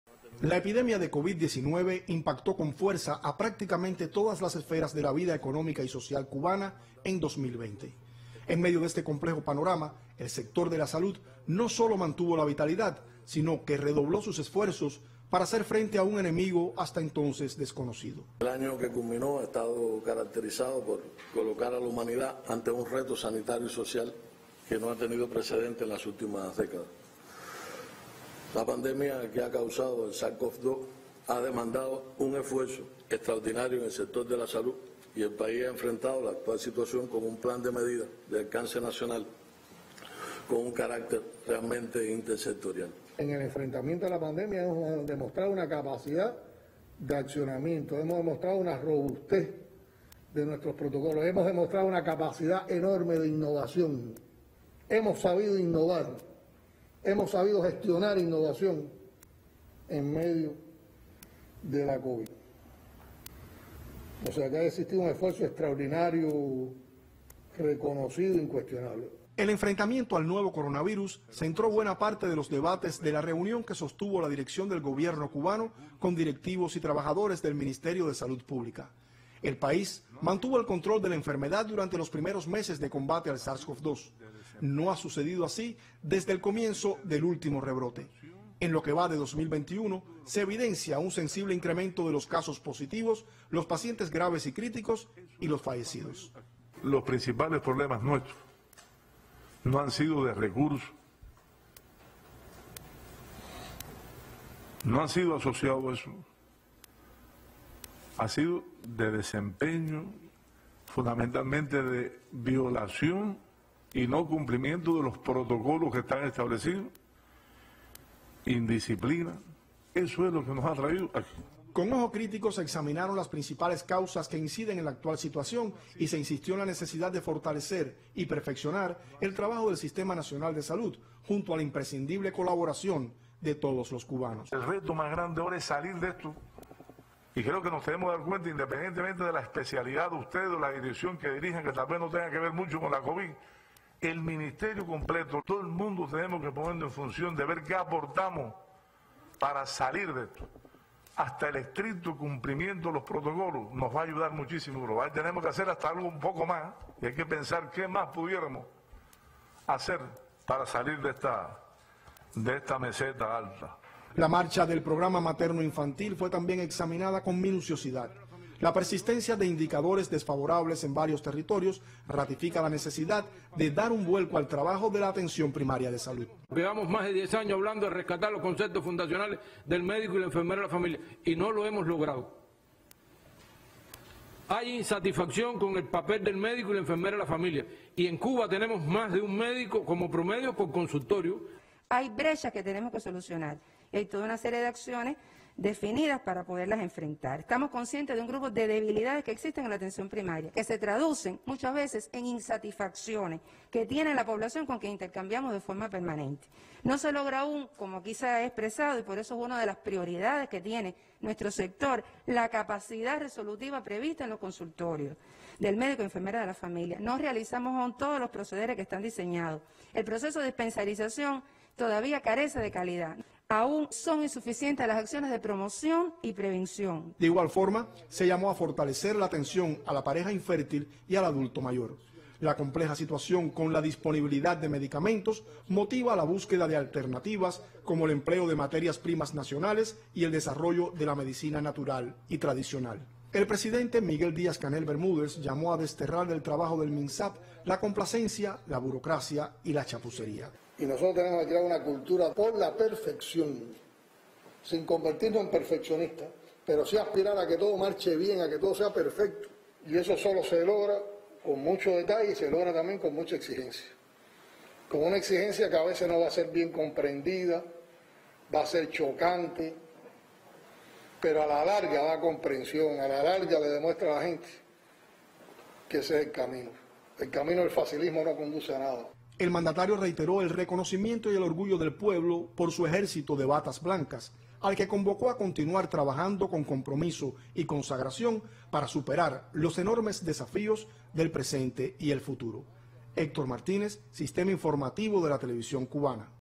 MINSAP de Cuba, retos y proyecciones para este 2021
A trabajar con compromiso y exigencia para enfrentar los desafíos del presente año, llamó el presidente de la República de Cuba, Miguel Díaz-Canel Bermúdez, a los directivos del Ministerio de Salud Pública (MINSAP). El mandatario presidió, junto al primer Ministro, Manuel Marrero Cruz, la reunión sobre las proyecciones de trabajo del sector en 2021. Le acompañaron el viceprimer ministro, Doctor Roberto Morales Ojeda y el titular del MINSAP, Doctor José Ángel Portal Miranda.